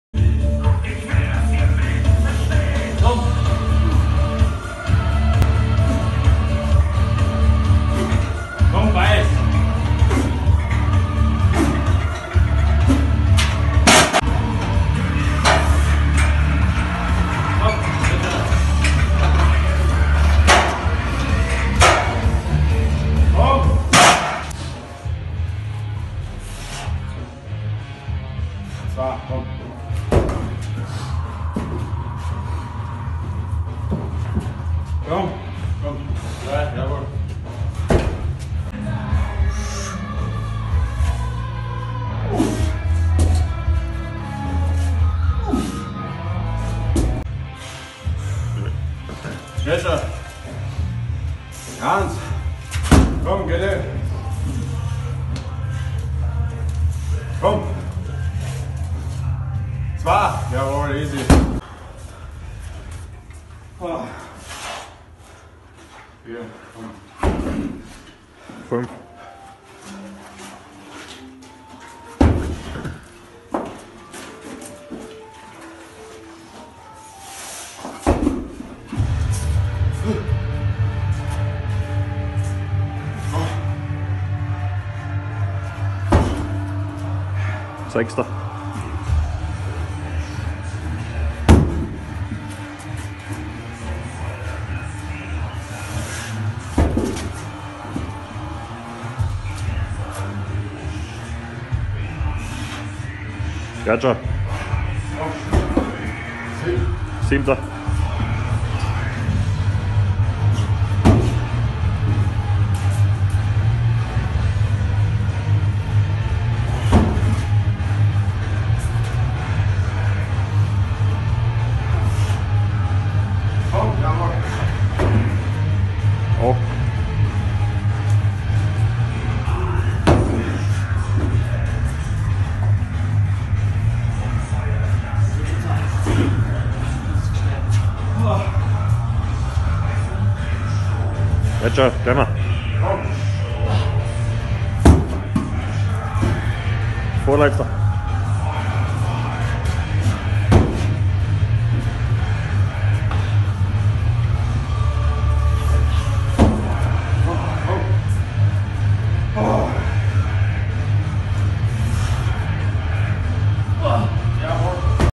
Strongman Training 25 02 2018
Ausschnitte vom Strongman-Training am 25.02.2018

Folgende Disziplinen wurden trainiert:
- Apollons Axle
- Yoke race
- Keg lifts